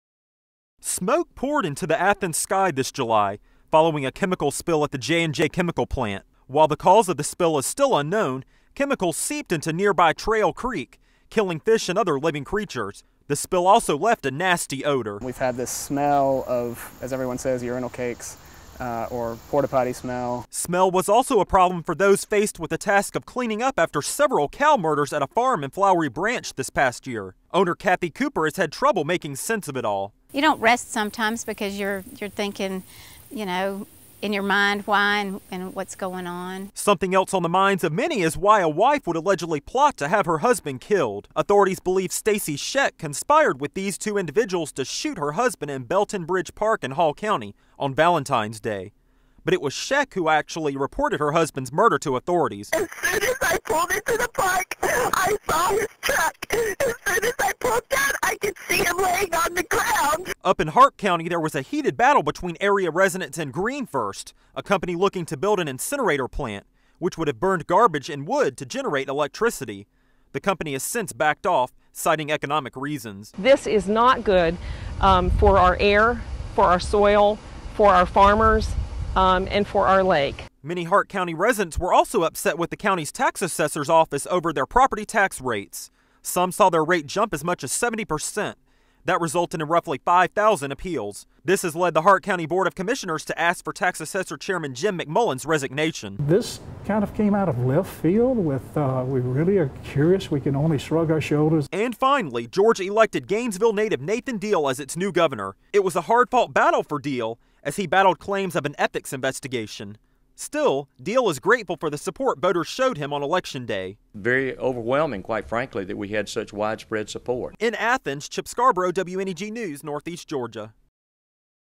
Recapping 2010 in Northeast Georgia
Smoke poured into the Athens sky this July, following a chemical spill at the J&J Chemical Plant. While the cause of the spill is unknown, chemicals seeped into nearby Trail Creek and killed fish and other living creatures.